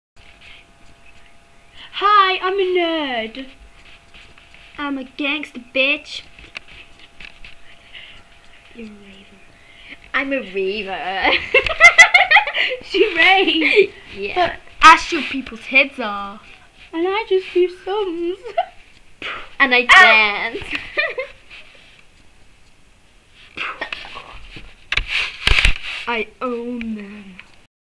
Emily-'I Owned You' :L
Lauren=NERD! , Chantal=Raver. Emily=Chav! At the end Emily owns chan and lauren by shooting them:L